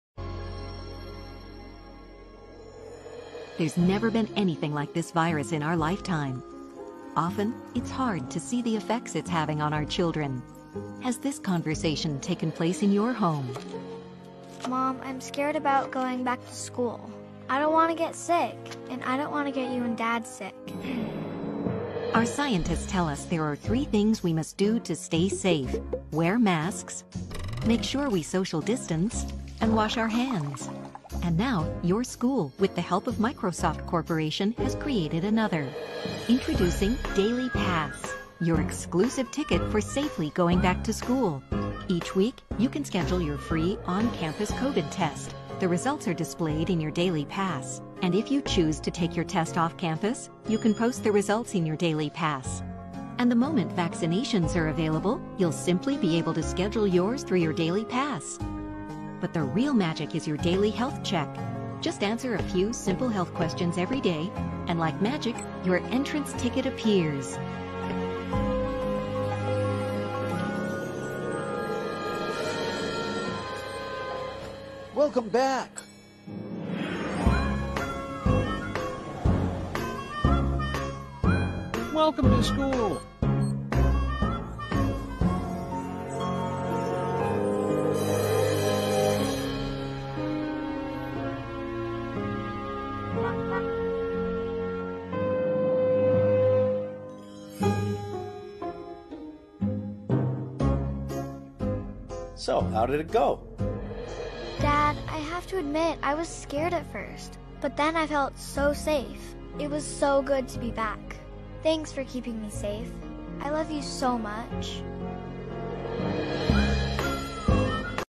A cartoon for children - California's COVID-19 Testing Program (Daily Pass)
Home schooling is the only way out.

Los Angeles Unified has launched a one-stop shop web app built specifically for the District to ensure that we get students, faculty and administrators back to schools and district offices as safely as possible. In addition to completing a daily health check through the app, users will be able to book COVID-19 tests and receive test results and messages.

Since when does Microsoft get exclusive access to the details of children?
When did these corporations get access to confidential medical records?
This link can be found on their login page that the children will use to sign in.

(you give them your content to use for marketing purposes)

Don't bother scanning the QR code unless you like Rick Astley.